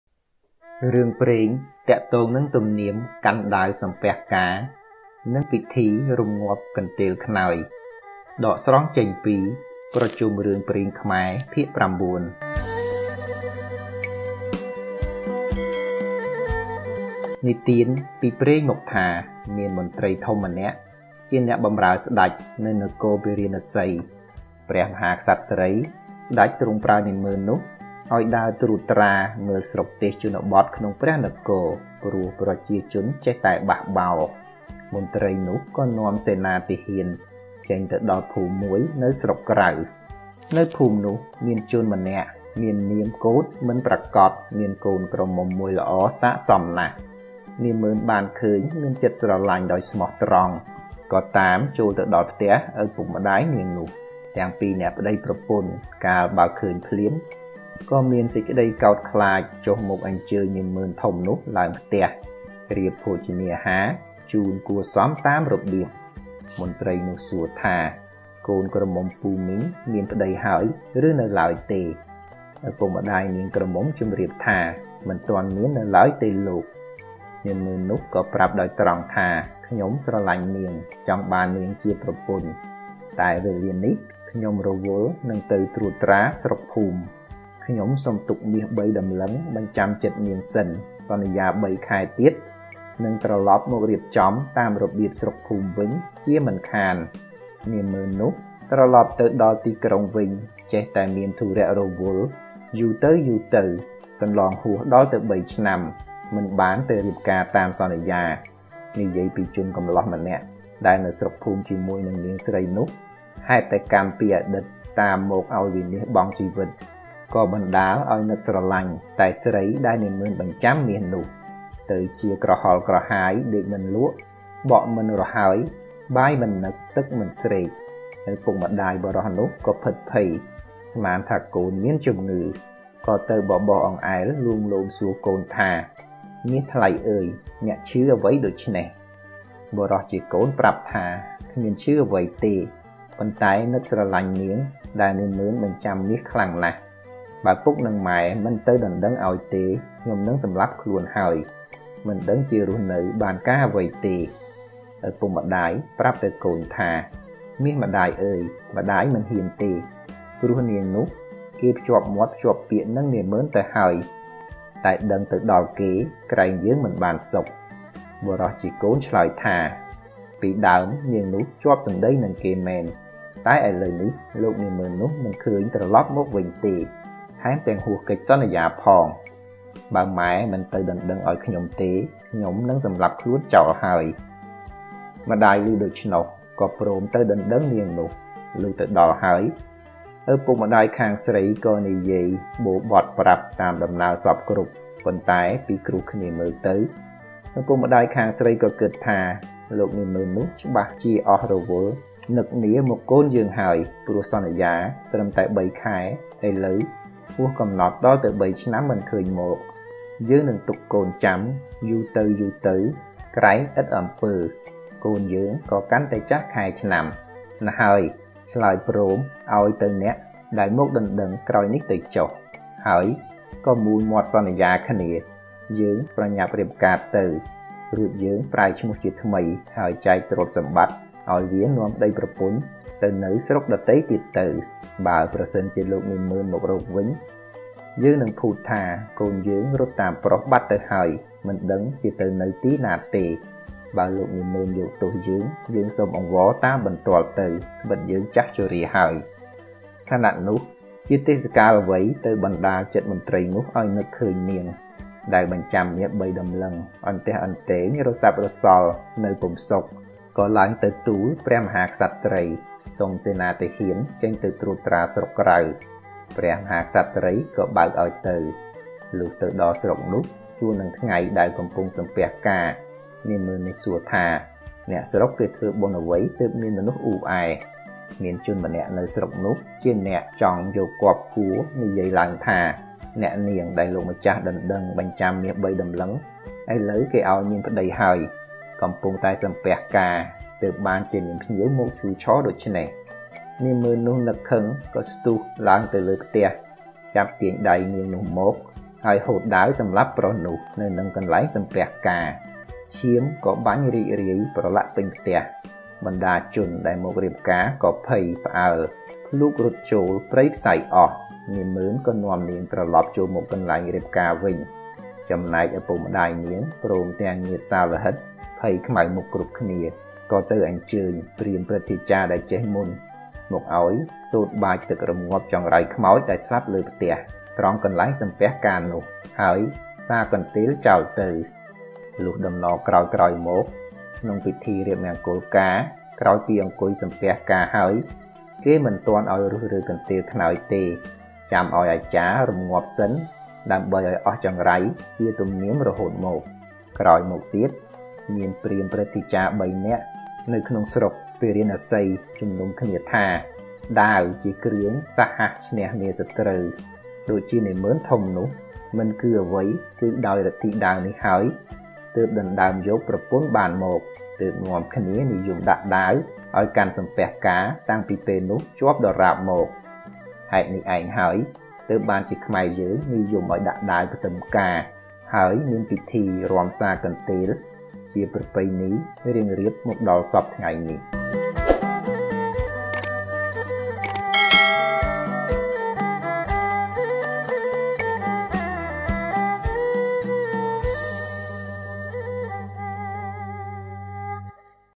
ទំនៀមកាន់ដាវសំពះការ និង ពិធីរម្ងាប់កន្ទេលខ្នើយ - ប្រជុំរឿងព្រេងខ្មែរ ២២៨
រឿងព្រេងទាក់ទងនឹងទំនៀមកាន់ដាវសំពះការ និង ពិធីរម្ងាប់កន្ទេលខ្នើយ (ដកស្រង់ចេញពីប្រជុំរឿងព្រេងខ្មែរ ភាគ ៩)

សុំមើលវីឌីអូខាងក្រោមនេះដើម្បីសិក្សាបន្ថែម៖